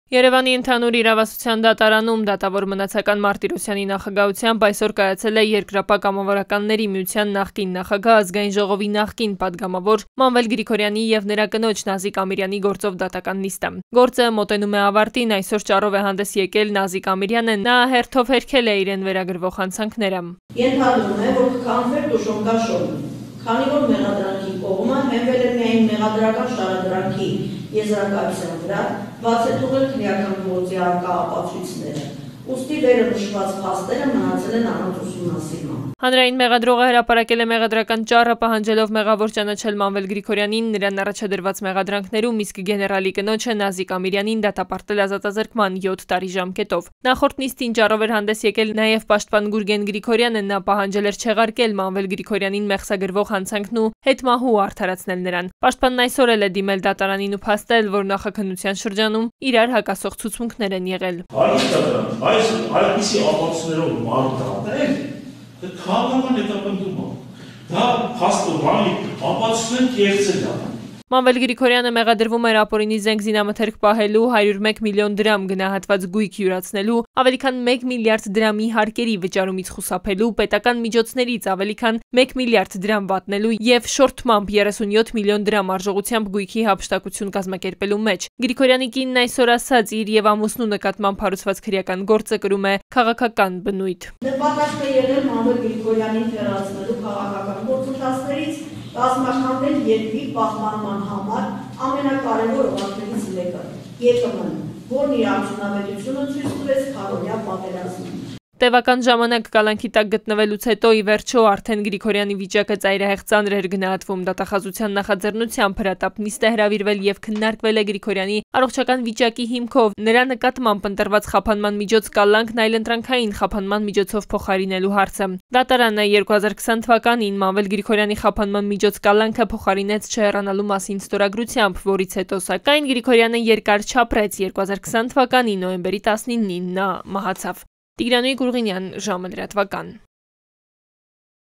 Մանվել Գրիգորյանի և Նազիկ Ամիրյանի գործով դատական նիստը մոտենում է ավարտին
Գեներալ Մանվել Գիգորյանի և Նազիկ Ամիրյանի գործով դատավարությունը մոտենում է ավարտին: Նազիկ Ամիրյանն այսօր հայտարարել է, ինքն անմեղ է, բոլոր մեղադրանքները կեղծ են, քրեական գործն էլ քաղաքական աստառ ունի: